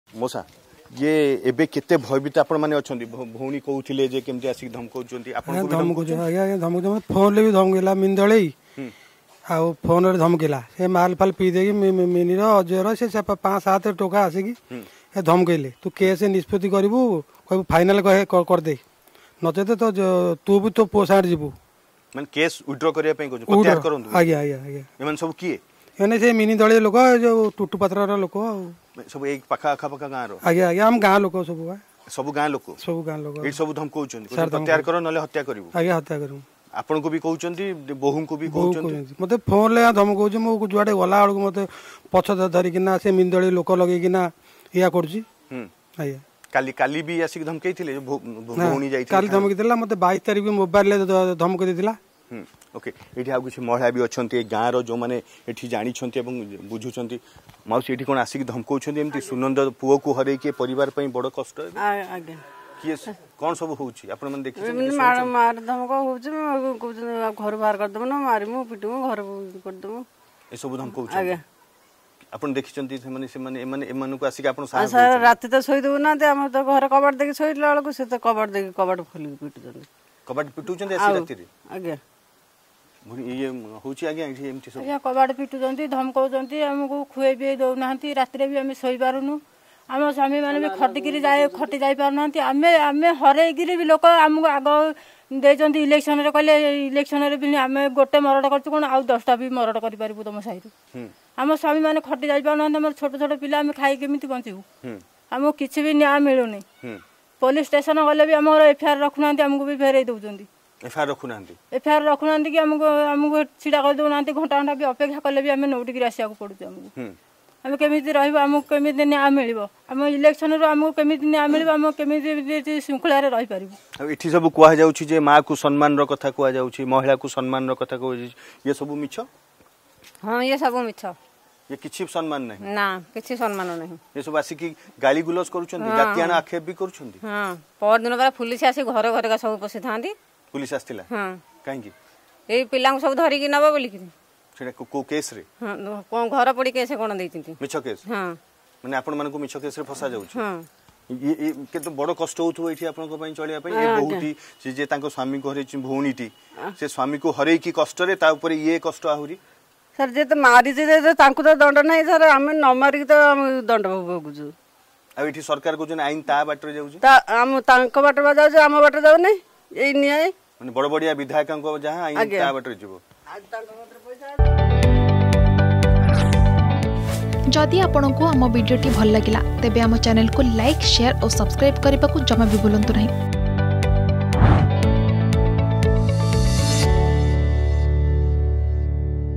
Tutu Patra's Goons Are Threatening Us: Sunanda's Father
"ଟୁଟୁ ପାତ୍ର ଲୋକ ଘରେ ପଶି ଡରାଇ ଜୀବନରୁ ମାରିଦେବାକୁ ଧମକ ଦେଉଛନ୍ତି" ||Tutu Patra||Justice For Sunanda Sethi